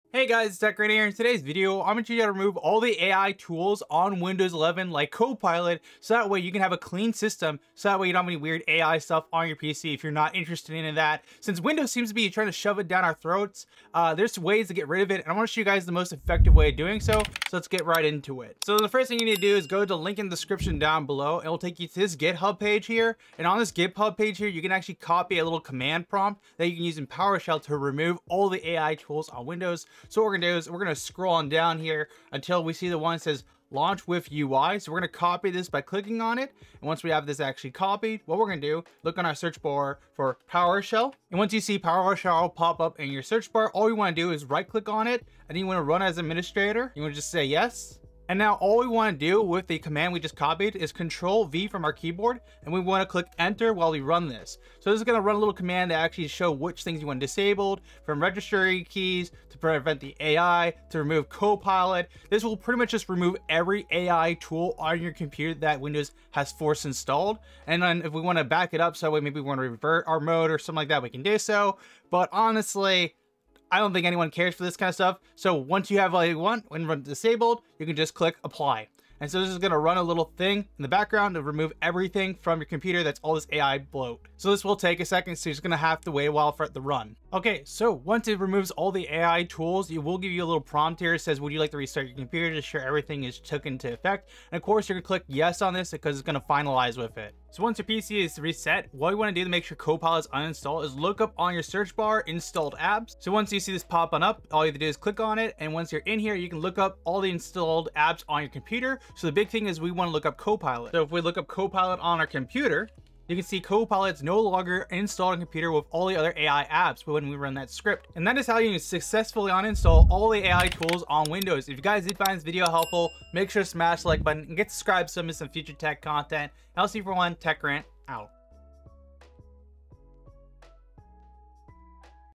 How to Remove All AI and Disable Copilot from Windows 11
Today I will show you how to remove all AI from windows 11 and disable windows copilot.

📦 All purchases for yourself will directly support my YouTube at no cost!